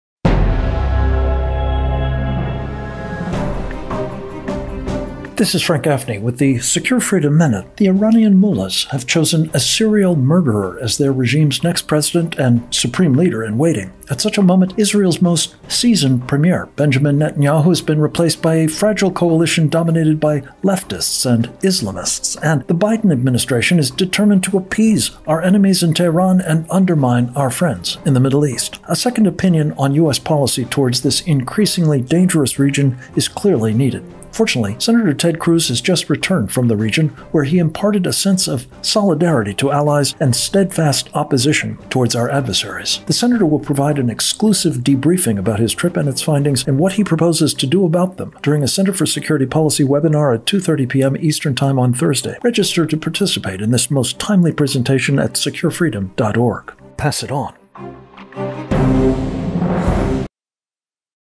Sen. Cruz to offer a second opinion on US Mideast policy
The Iranian mullahs have chosen a serial murderer as their regime’s next president and Supreme Leader-in-waiting. At such a moment, Israel’s most seasoned premier, Benjamin Netanyahu, has been replaced by a fragile coalition dominated by leftists and Islamists.